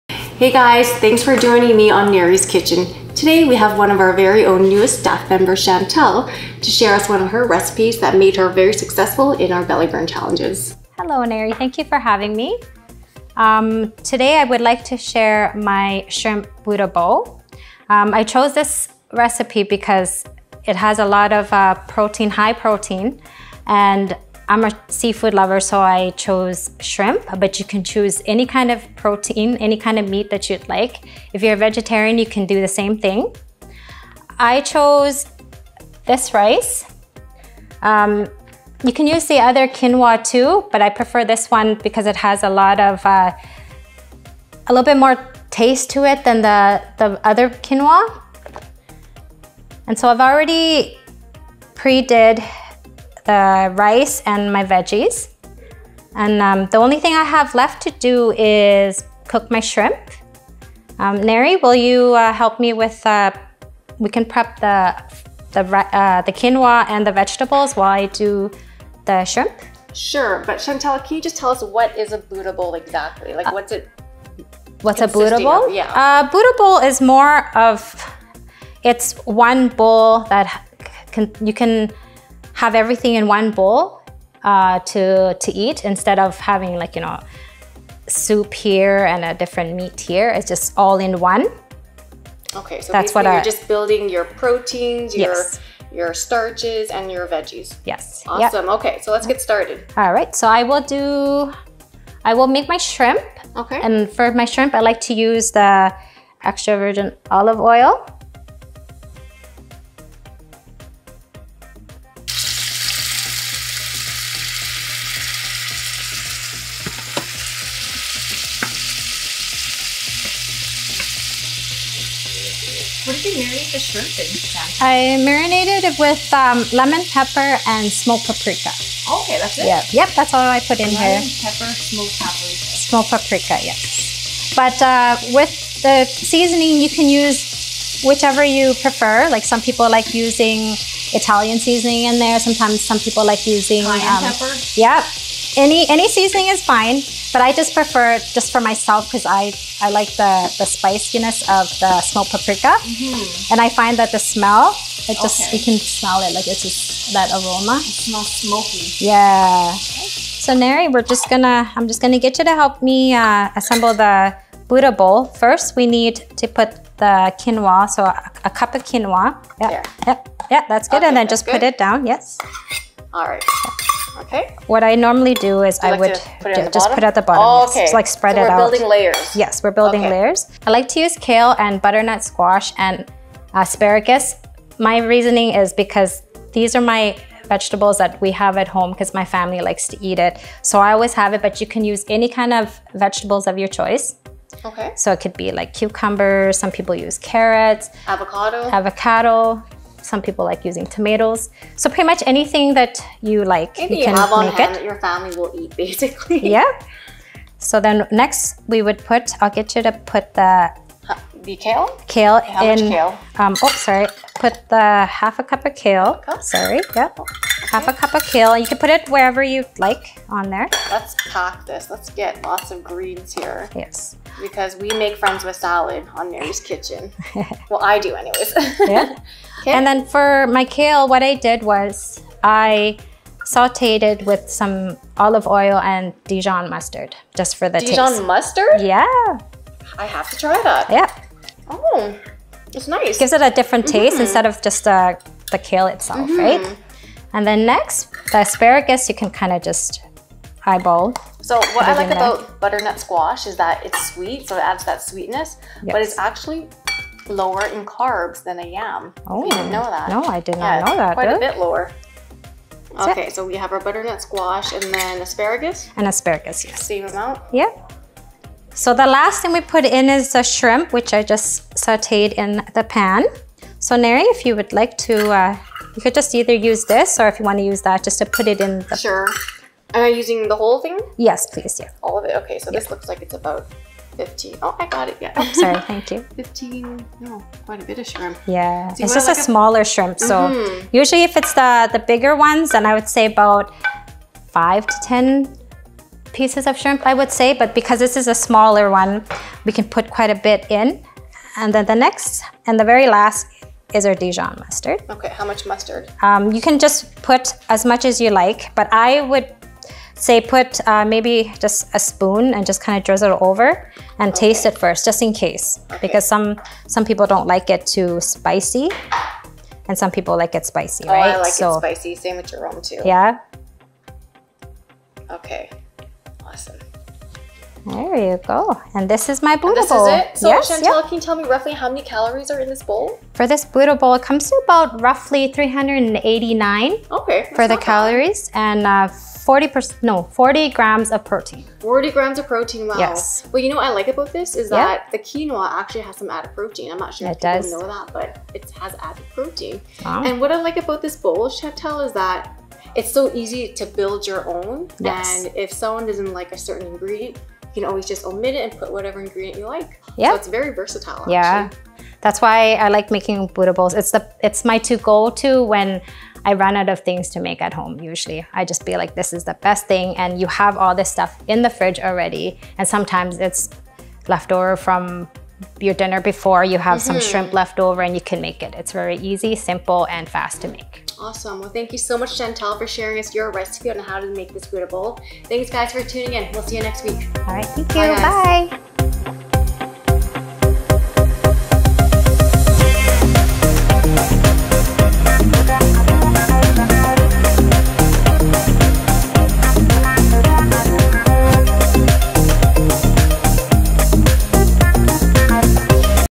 Low Carb Shrimp Buddha Bowl
A combination of veggies and flavors that we love to enjoy in our home. This recipe was a mix of veggies consisting of chopped fresh kale, roasted butternut squash and asparagus, and quinoa.

The sky is endless for this Buddha Bowl recipe.
It can be whatever you want it to be, with whatever combination of flavors you wish!